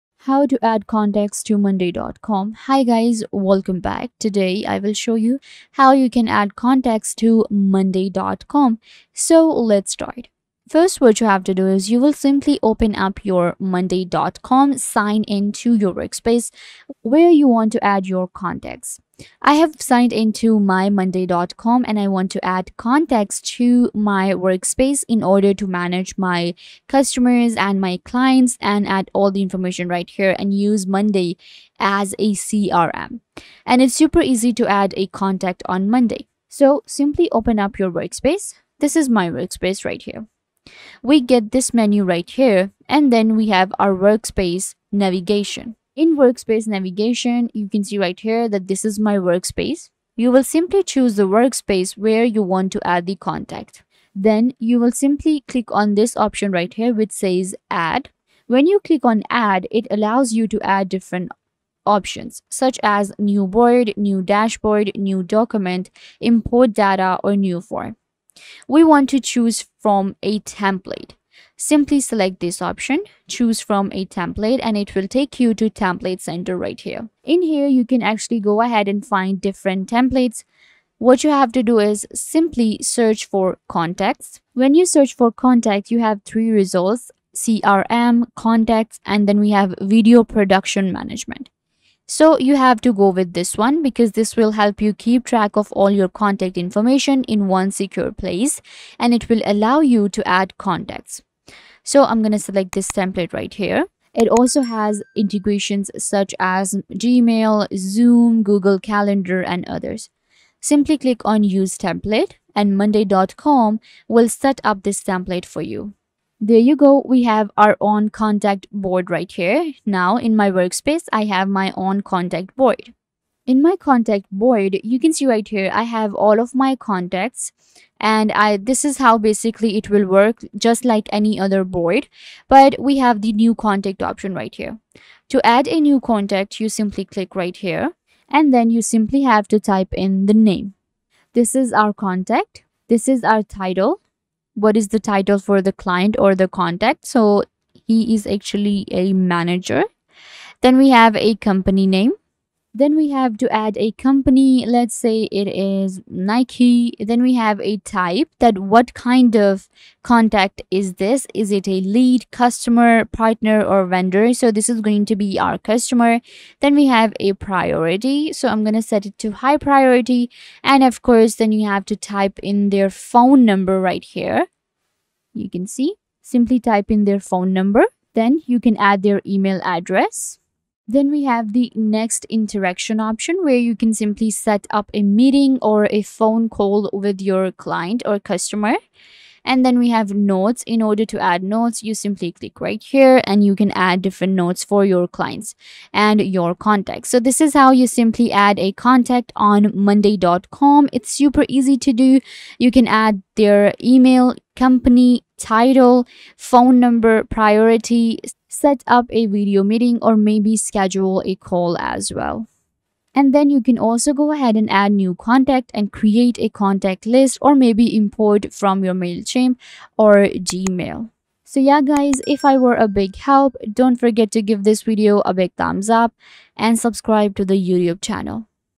How To Add Contacts To Monday.Com | Step By Step Tutorial (2025)
How To Add Contacts To Monday.Com

In this video I show you How To Add Contacts To Monday.Com. This is super easy and you can do it in just a few minutes by following this helpful tutorial.